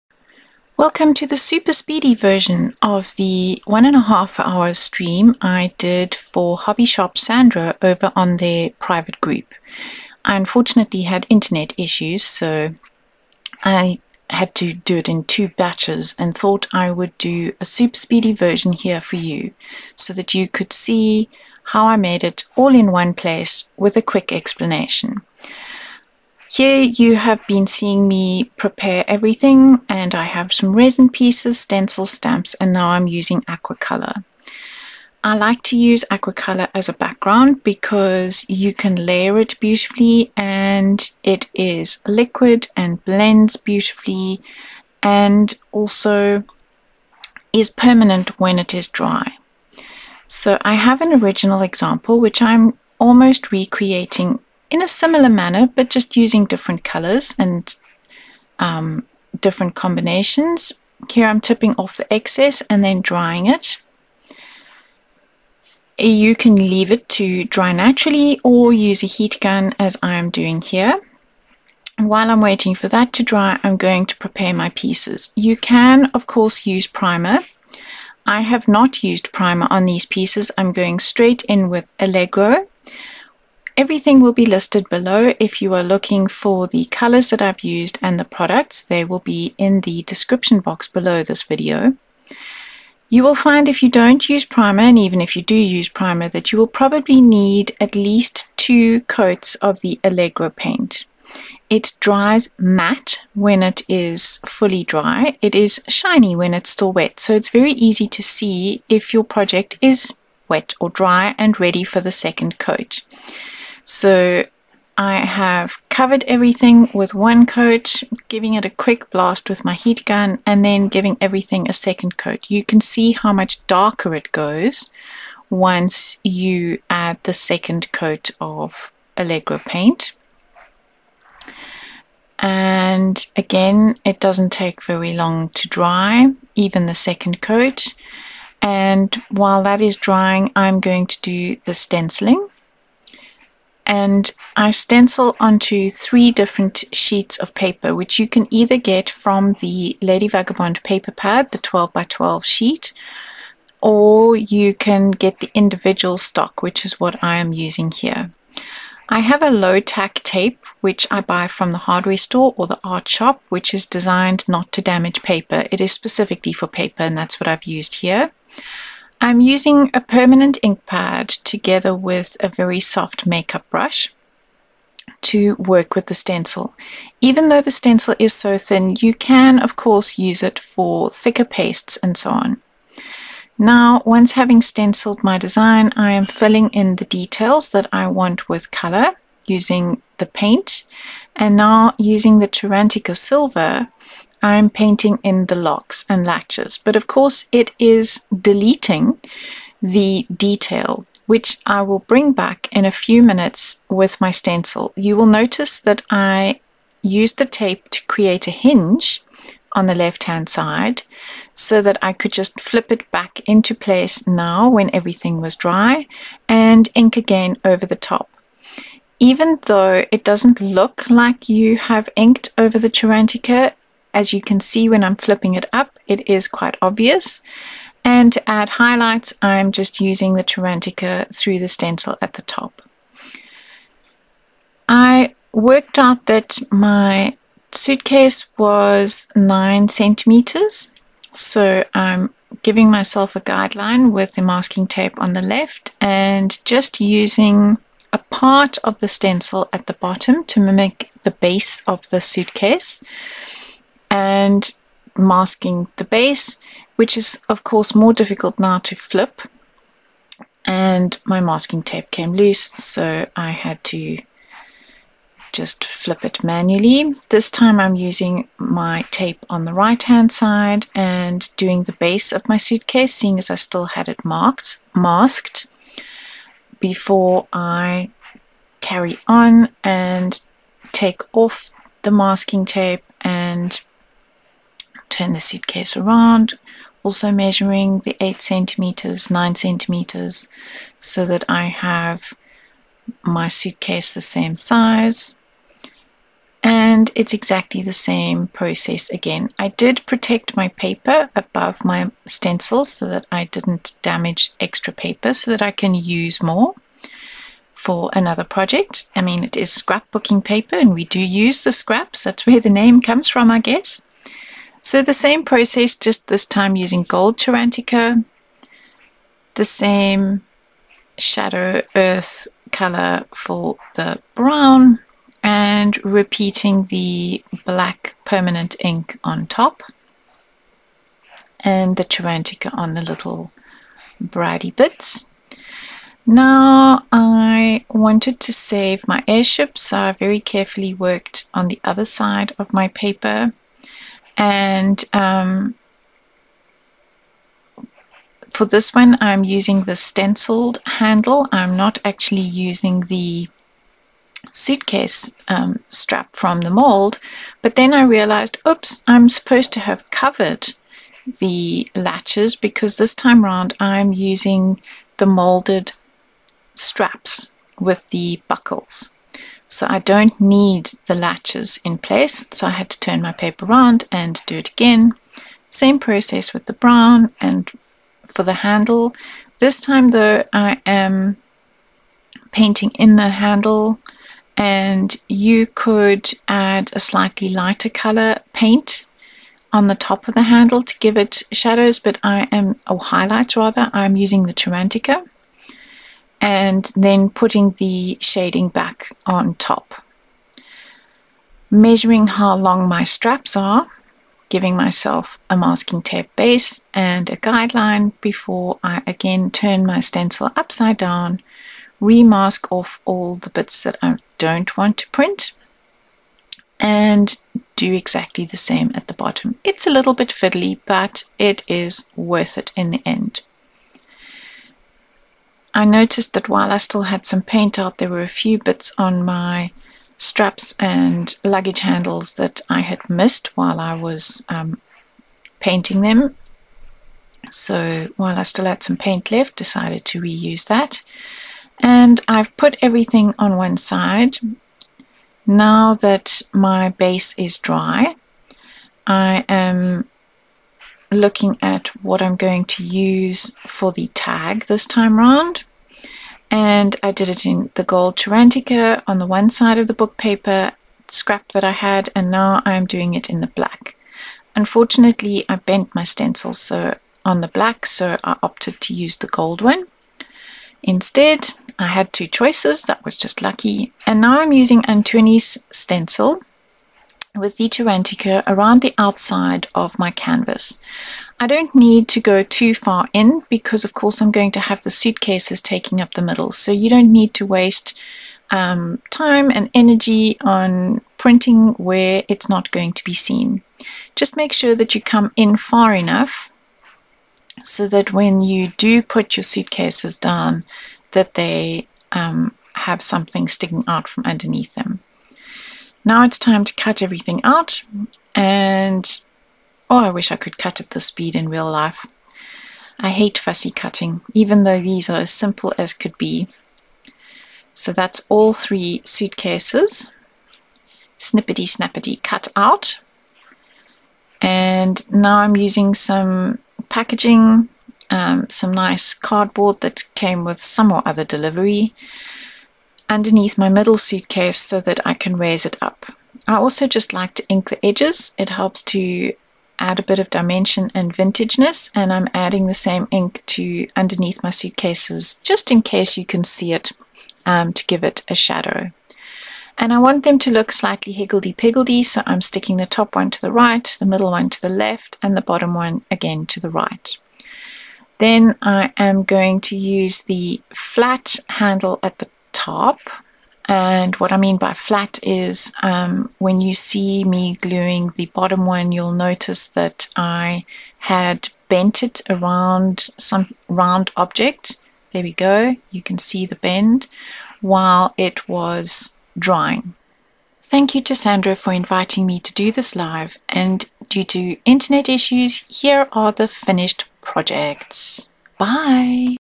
Suitcase Canvas
This is the fast version of a Facebook live I did for Hobbyshop Sandra.

Unfortunately I had internet issues on the day, so decided to upload it here as a fast version.  Due to the internet dying on me again at the end, you do not see me glue everything down... but there is a photo to show the original and the finished canvas from this live at the end.
Products used:
Aqua Color Aquamarine KAQ015
Aqua Color Turquoise KAQ018
Allegro Earth KAL106
Cerantica Silver K3P15S
Cerantica Gold K3P15G
Papers SBB760, SBB759, Bb757
Mould K3PTA484 Locks
Stencil KSG455 Luggage
Stencil KSAT15 Wallpaper and Compass